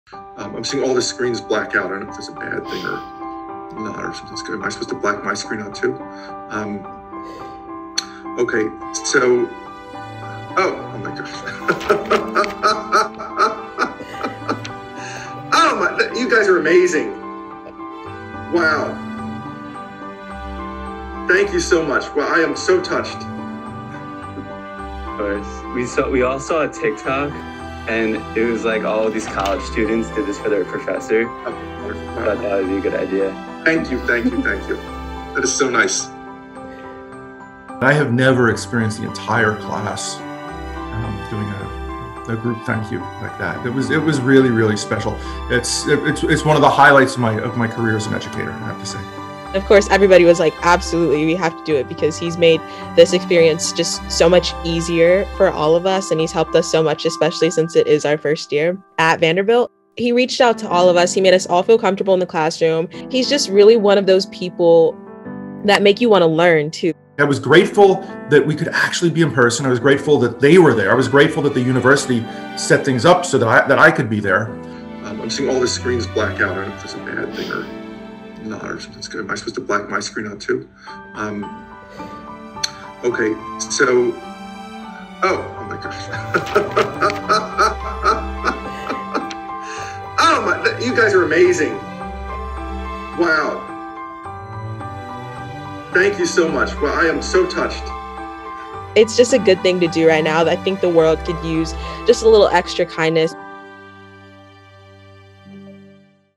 Students surprise a professor with a special act of gratitude
A Vanderbilt professor received a heartwarming surprise from his class of mostly first-year students. On the last day of class, Shaul Kelner, associate professor of sociology and Jewish studies, started seeing his student’s Zoom screens go black.